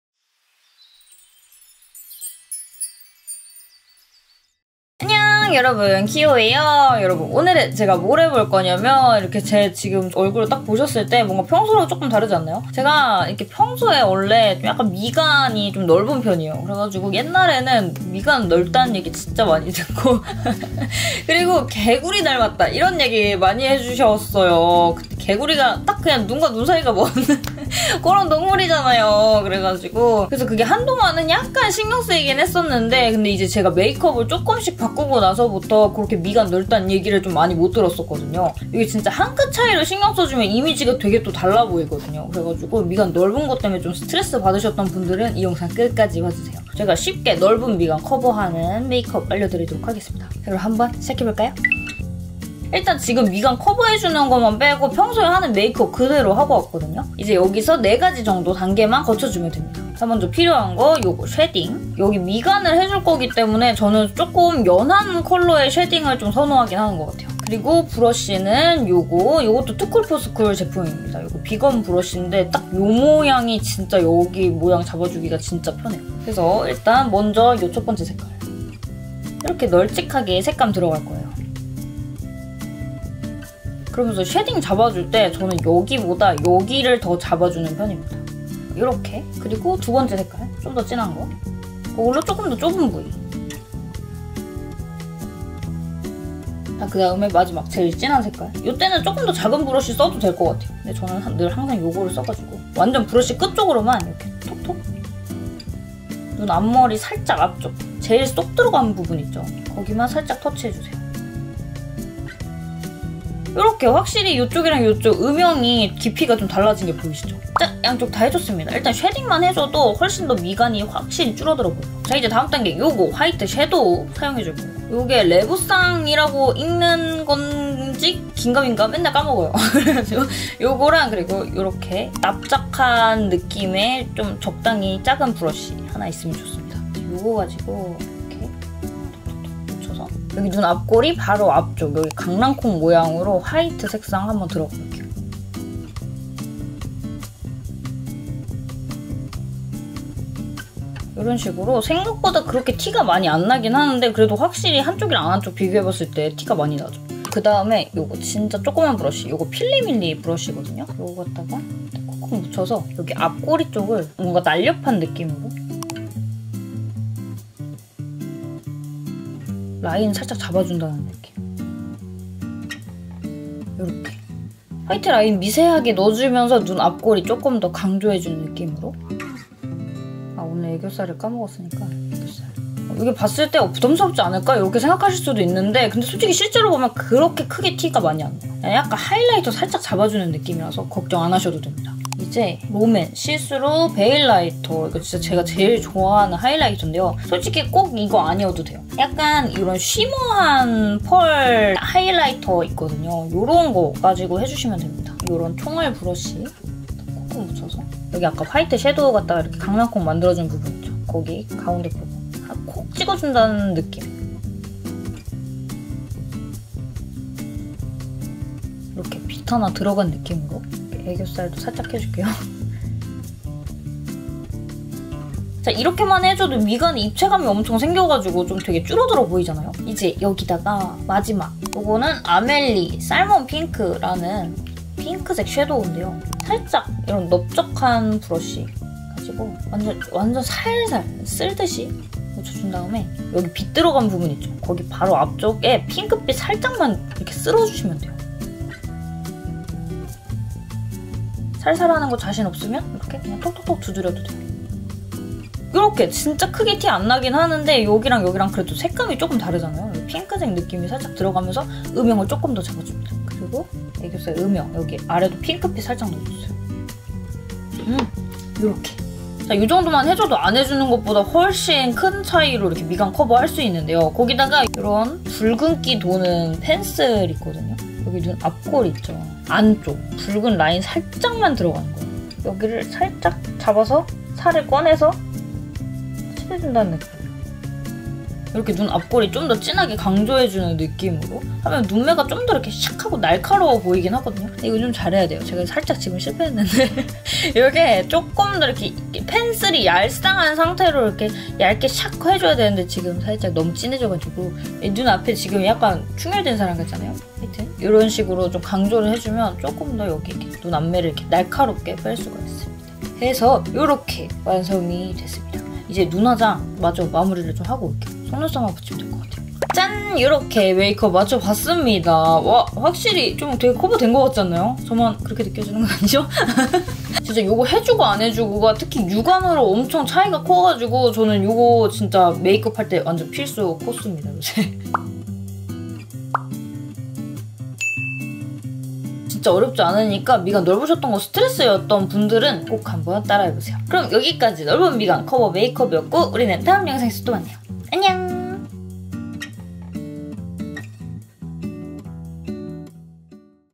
착시효과 100% 넓은 미간 성형 메이크업! 시원한 앞트임 효과까지!
[제품 정보]

01:00
투쿨포스쿨
아트클래스 바이 로댕 쉐이딩

투쿨포스쿨
비건 브러쉬
페이스 포인트 브러쉬

02:02
REVECEN
베이직컬러 아이섀도우

필리밀리
아이브러시 프로컬렉션
#02 포인트

필리밀리
#05 언더

03:12
롬앤
시스루 베일라이터
#01 선 키스드 베일

필리밀리
미니 메이크업 브러시세트
블렌딩 섀도우 브러시

03:51
아멜리
스텝베이직
#229 살몬핑크

필리밀리
#01 베이스

04:35
에뛰드
플레이 101 펜슬
#15 (단종)